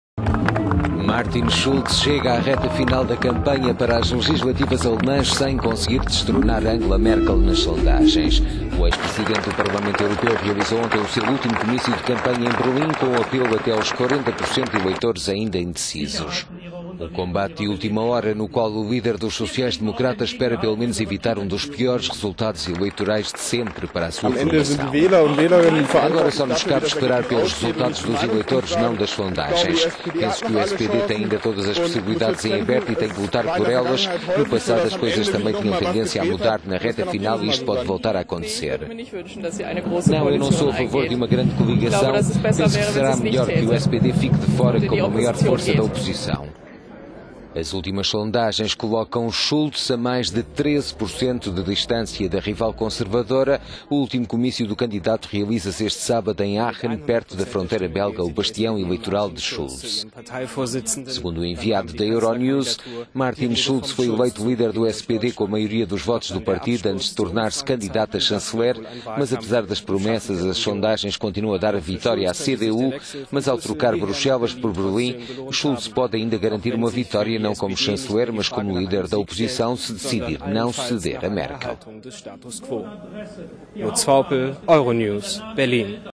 Martin Schulz apela a indecisos na reta final da campanha
Martin Schulz chega à reta final da campanha para as legislativas alemãs sem conseguir destronar Angela Merkel nas sondagens. O ex-presidente do parlamento europeu realizou ontem o seu último comício de campanha em Berlim, com um apelo aos até 40% de eleitores ainda indecisos.

Um combate de última hora, no qual o líder dos Sociais-Democratas espera pelo menos evitar um dos piores resultados eleitorais para a formação nos últimos anos, quando recolhe apenas 21% das intenções de voto, a mais de …